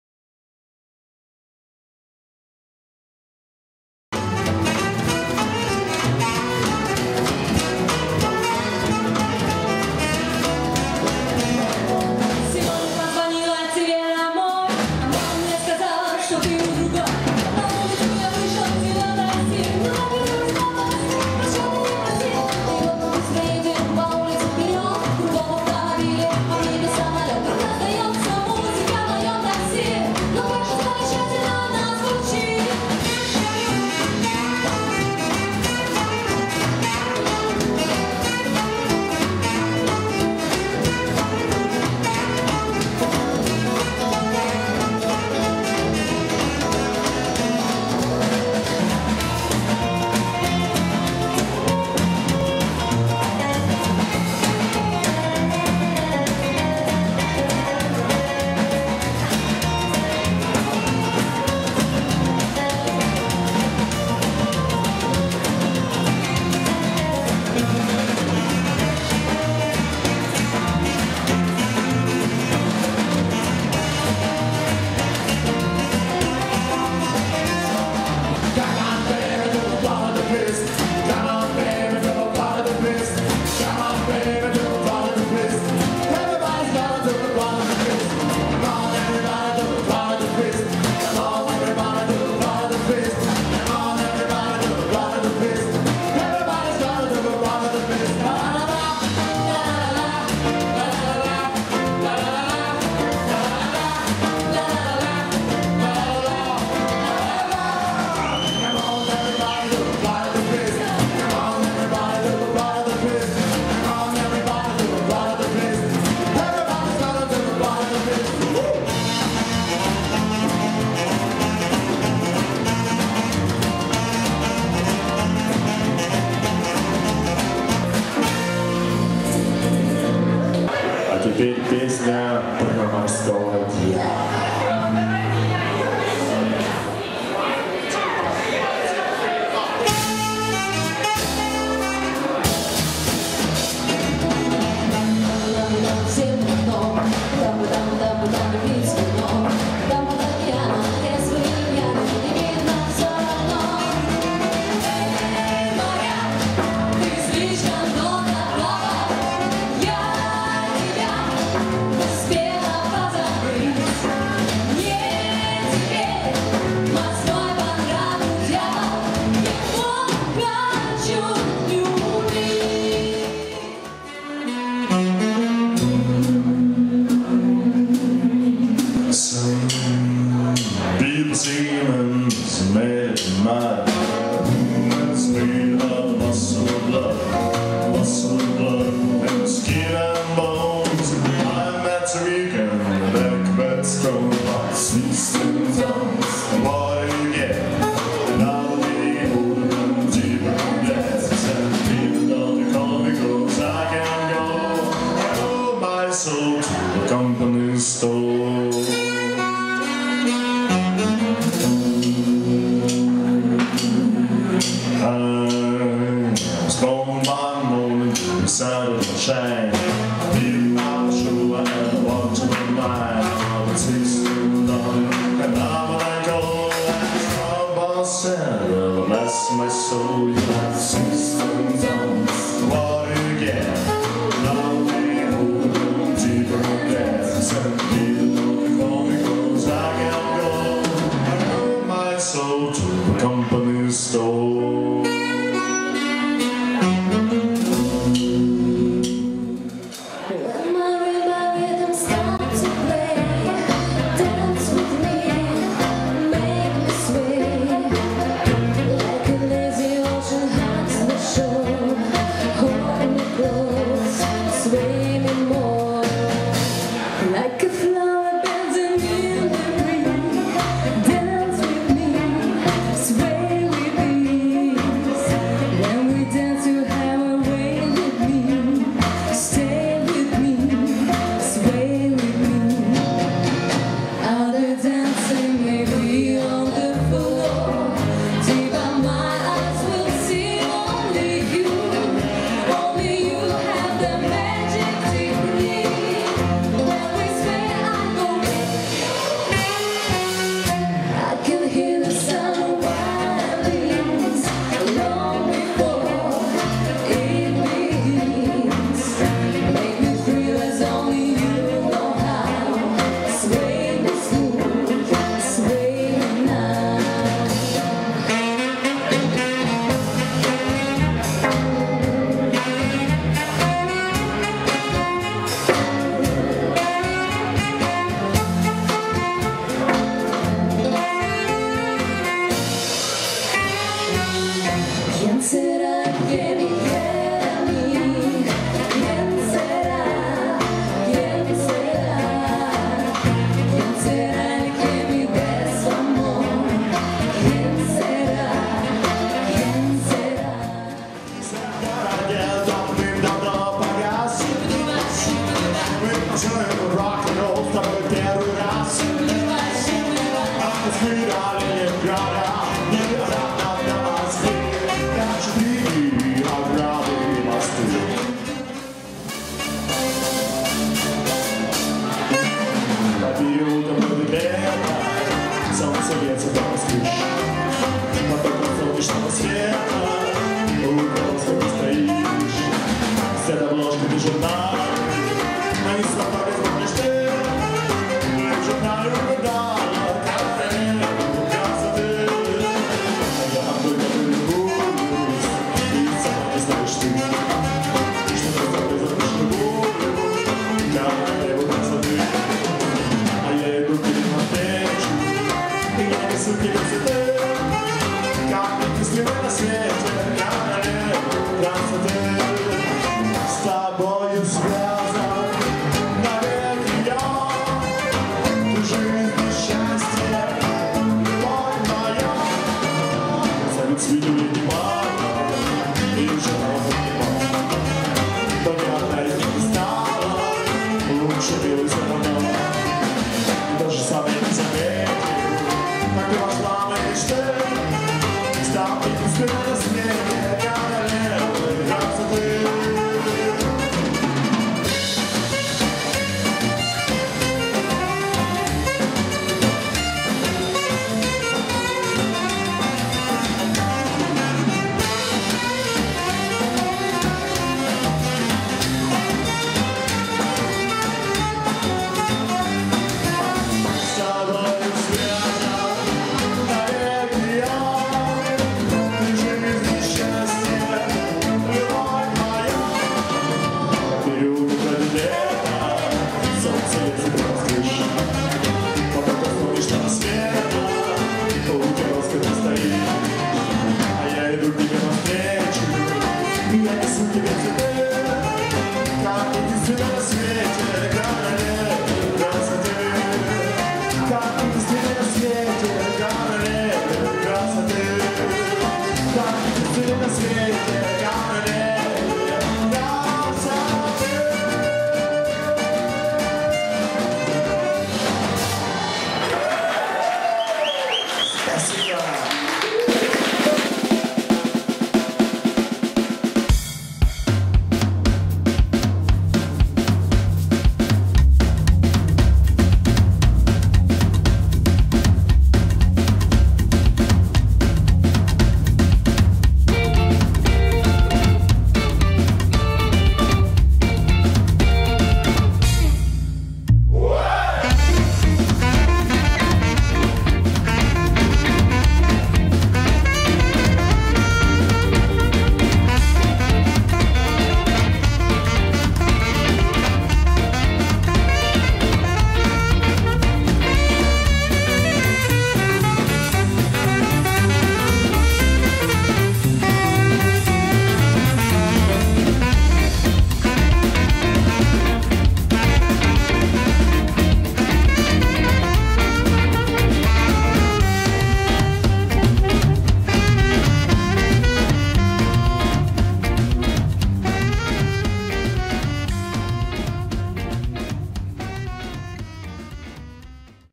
LAS-VEGAS COMBO
Демо-ролик LAS-VEGAS COMBO : фрагменты концертного выступления в клубе-ресторане НаBeerЕжная,фотографии,контакты.